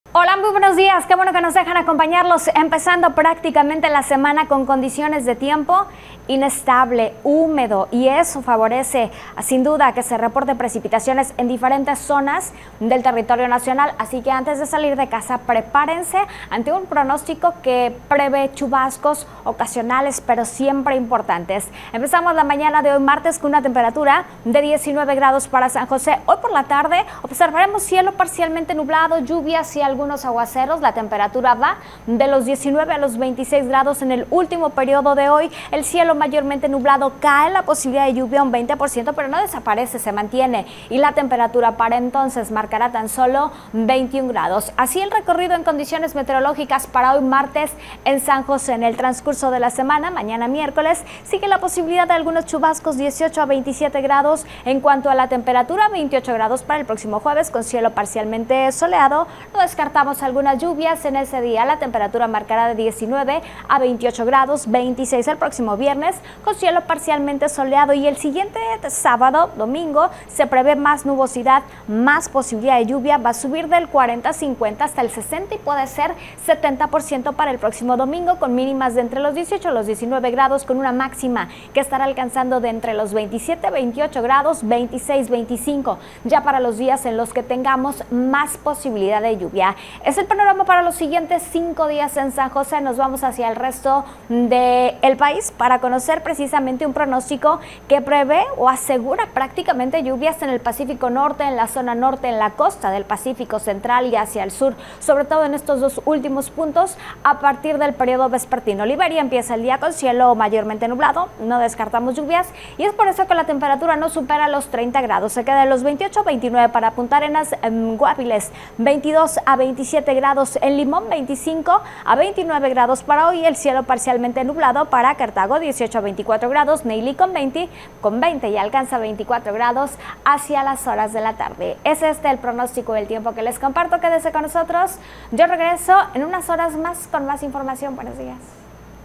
Pronóstico del tiempo para Costa Rica 27 de junio de 2023, con Emily Quiñones
Emily Quiñones nos da el pronóstico del tiempo en Costa Rica para este martes 27 de junio de 2023.